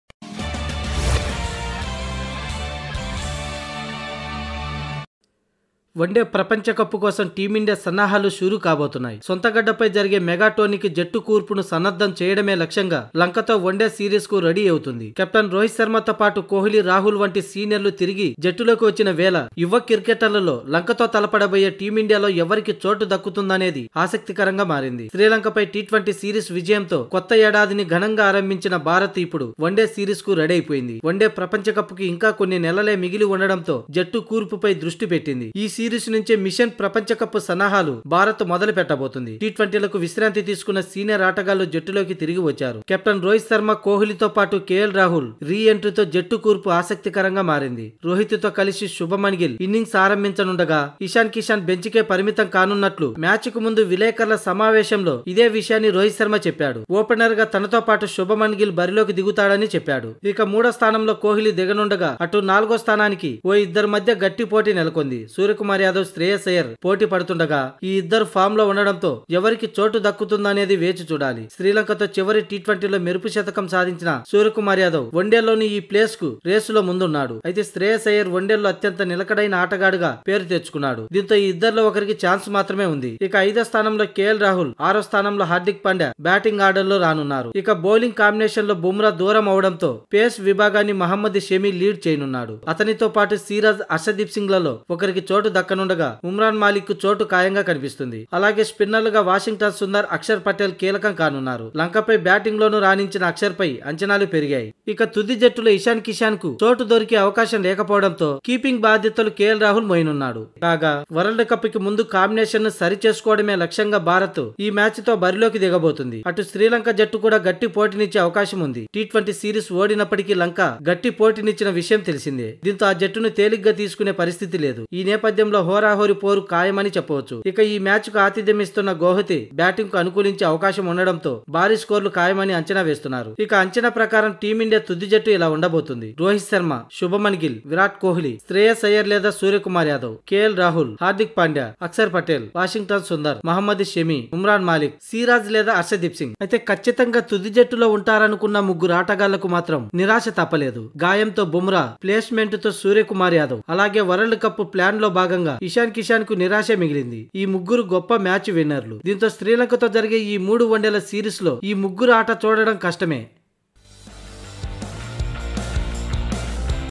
Three Key Changes in India Playing XI for India vs Sri Lanka 1st ODI in Guwahati || Ind vs Sl 2023
Three Key Changes in India Playing XI for 1st ODI vs Sri Lanka in Guwahati |Ind vs Sl 1st ODI